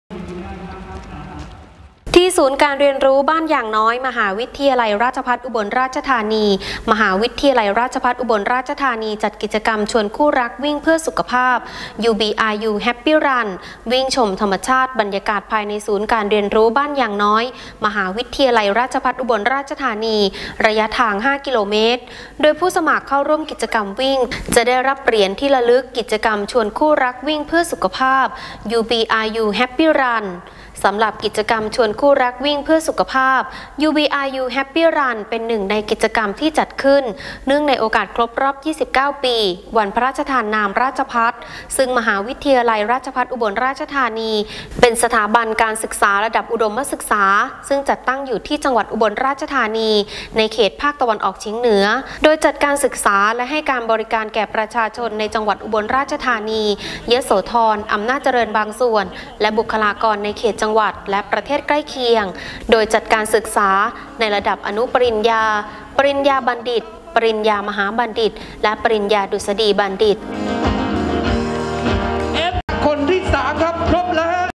ชวนคู่รักวิ่งเพื่อสุขภาพ UBRU HAPPY RUN
มหาวิทยาลัยราชภัฏอุบลราชธานี จัดกิจกรรมชวนคู่รักวิ่งเพื่อสุขภาพ “UBRU HAPPY RUN” วิ่งชมธรรมชาติบรรยากาศภายในศูนย์การเรียนรู้บ้านยางน้อย

รับชมเพิ่มเติมได้ทาง : เพจ สถานีข่าววีเคเบิล ทีวีอุบลฯ